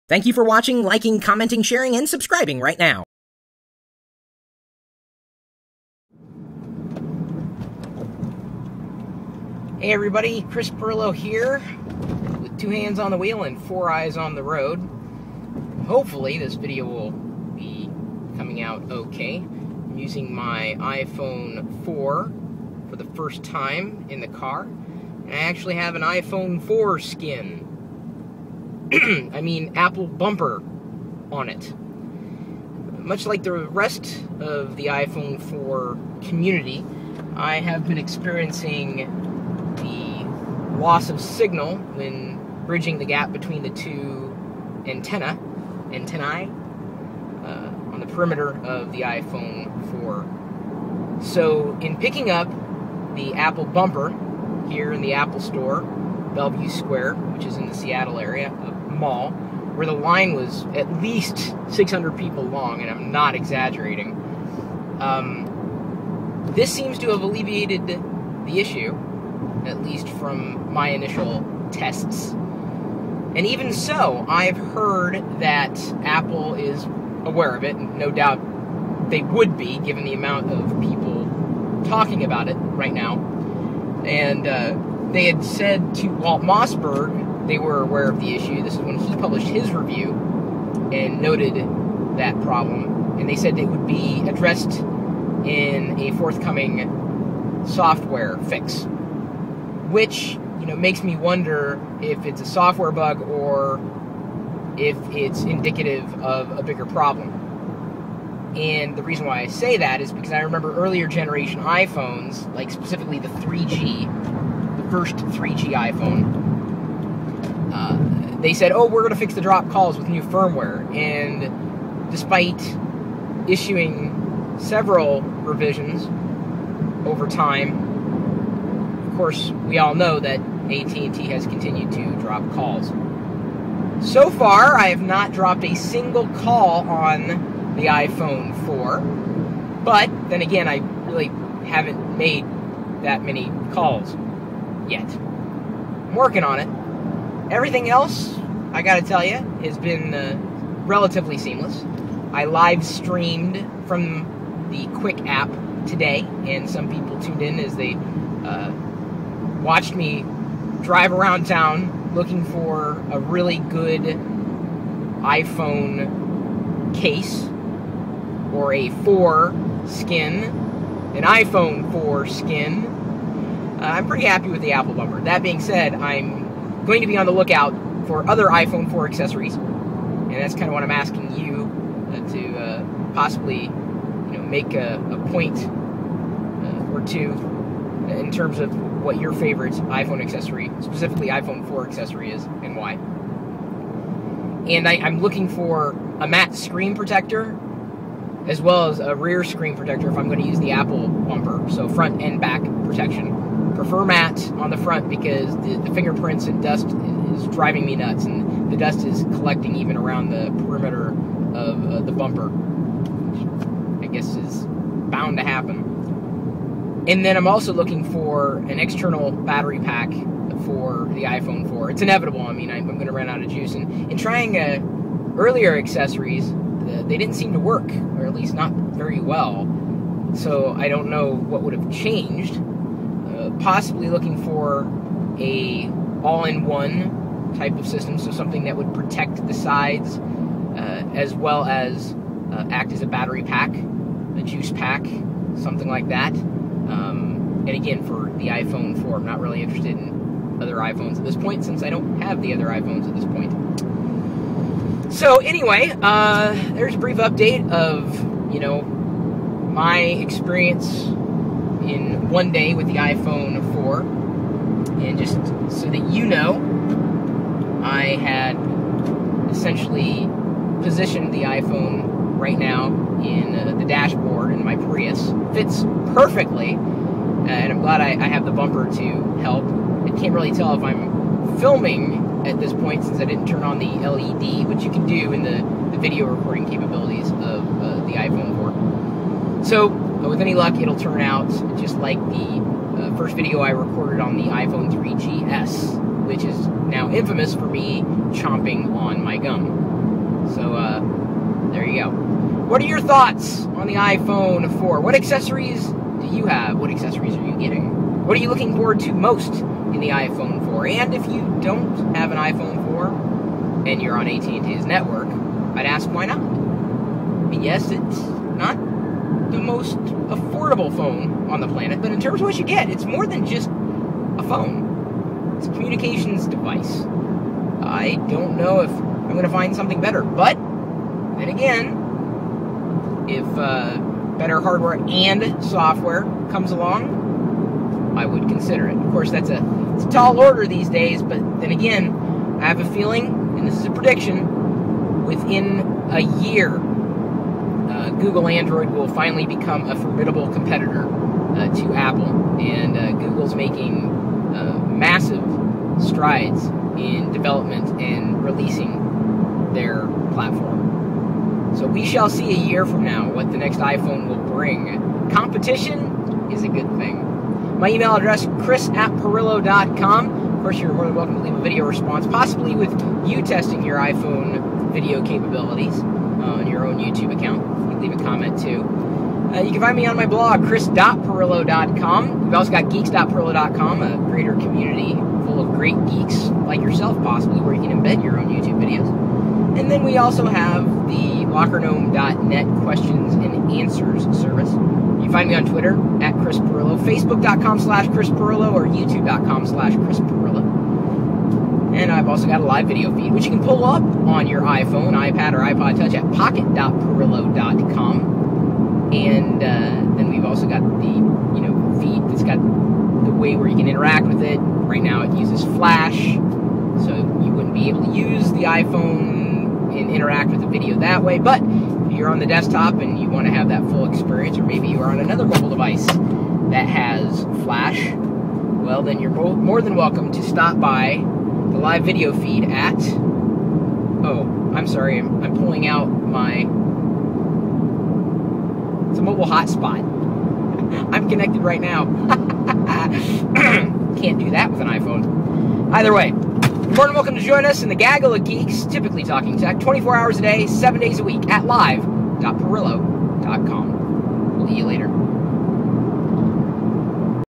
iPhone 4 Updates: Skins, Antenna Problems, iMovie App
- Well, it's been about a day since I've switched from the iPhone 3GS to an iPhone 4, and I thought I'd gather some of my thoughts in video - using the iPhone 4 while I was on the road.

The video came out very well, in my estimation - except I couldn't control where it was focused (and there was no way of telling how it was focused since I couldn't see the screen). One solution could have been to use the front-facing camera, but then the footage would have come out at a lower resolution.

I decided (at the last second) to edit the video in iMovie on the iPhone - if only to see how easy it was to do. Indeed, it was easy. Uploaded directly to YouTube from the app, too!

Still, the questions remain: what do you think about the iPhone 4 so far? What accessories are you getting? What would you recommend to the rest of the iPhone community?